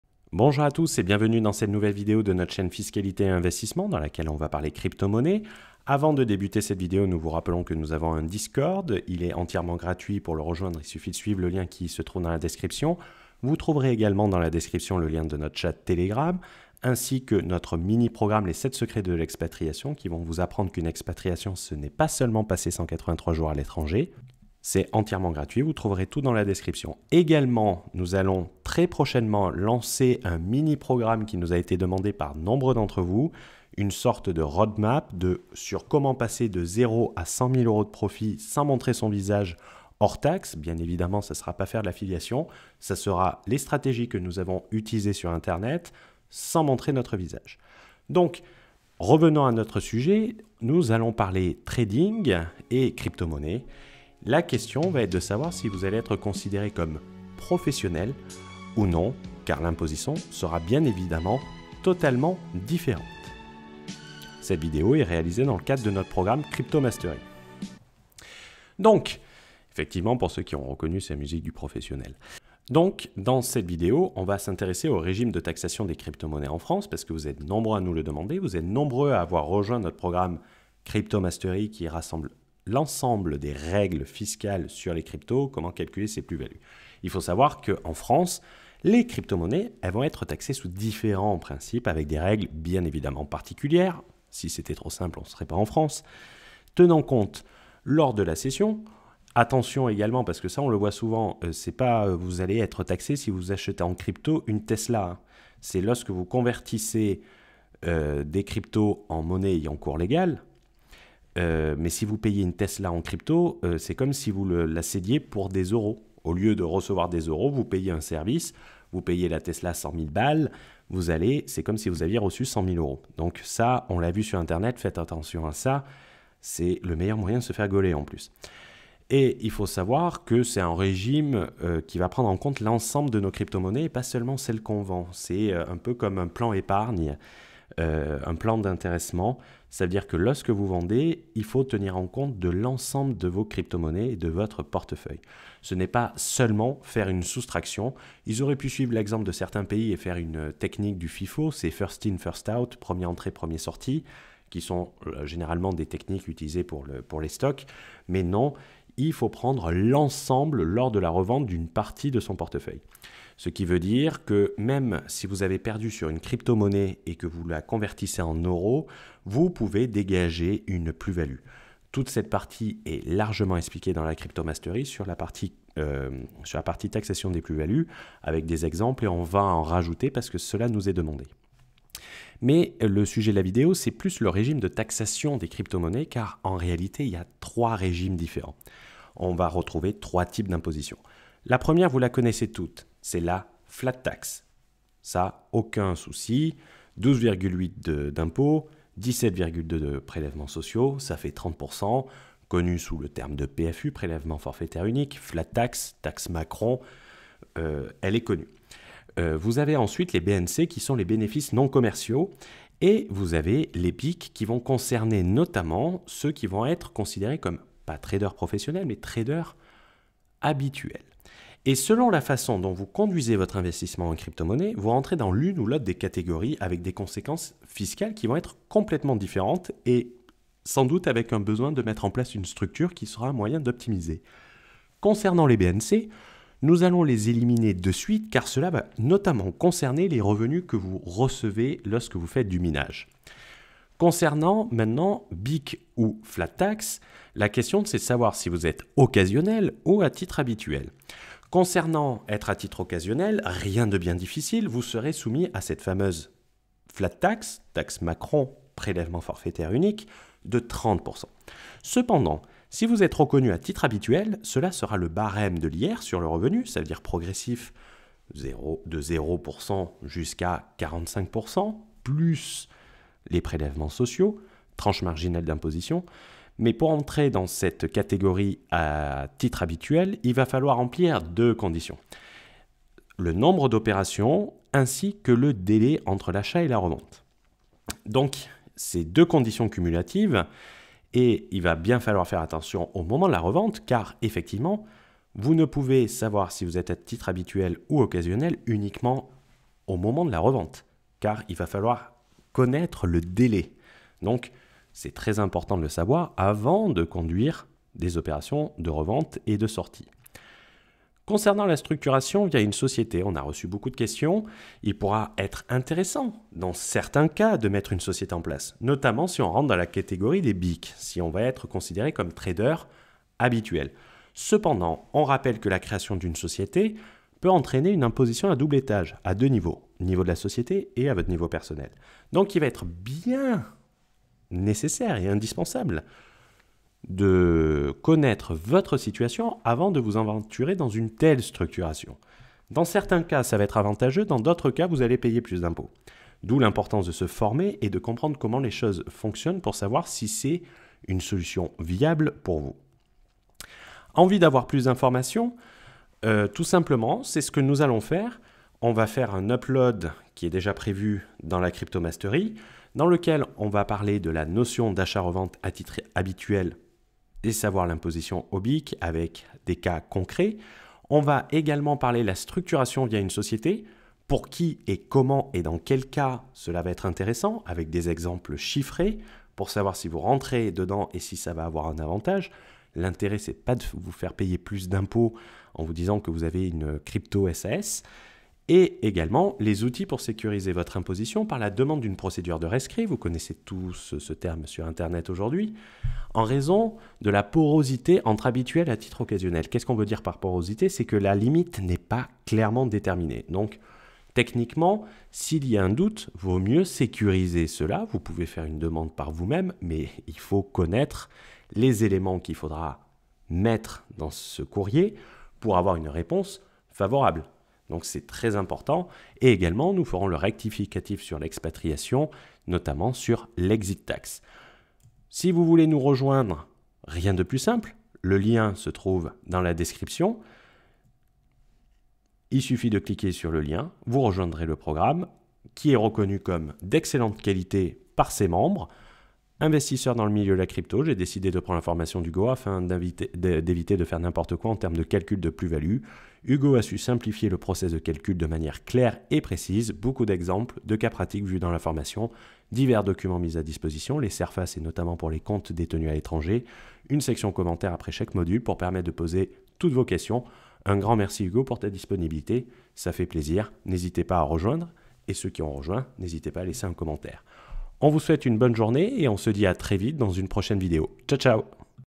CRYPTO : Imposition, Optimisation en FRANCE 🇫🇷
CRYPTO : Comment sont-elles imposées en France et comment pouvons nous optimiser ? Les BTC, ETH et autres Crypto connaissent une folle envolée ces derniers mois. Il est peut-être venu le temps de les vendre ? Mais comment allez vous être imposé ? C'est à cette question que nous répondons dans la vidéo.

⬇️ Infos en description : ⬇️
▬▬▬▬▬▬▬▬▬  NOS FORMATIONS   ▬▬▬▬▬▬▬▬▬▬

▬▬▬▬▬▬▬▬▬▬▬▬▬ Informations ▬▬▬▬▬▬▬▬▬▬▬▬